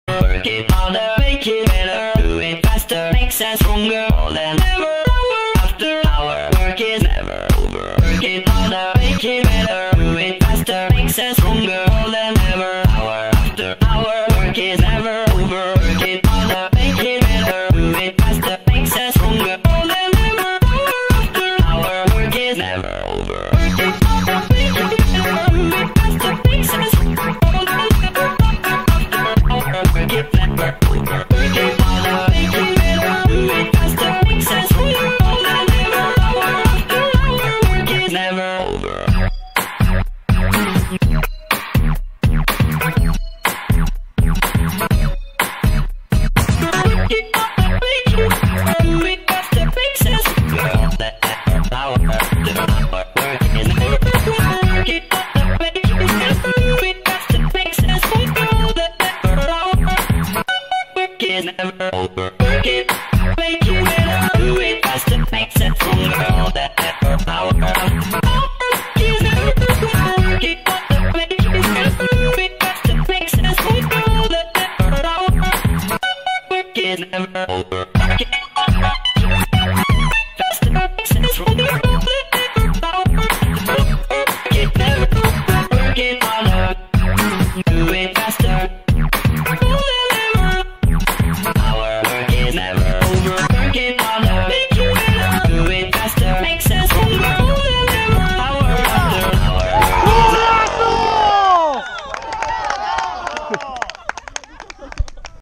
Big Victory for the Might Caballo Loco!!!
Highlights from today's Caballo Loco Football match. The team won 5 - 2, coming back from being 2 - 1 behind.

Key moments from this exclusive video include Toshi's second half strike and Nano's far post, bullet header.

Also, the music is not mine, sorry about that... I just really like Daft Punk.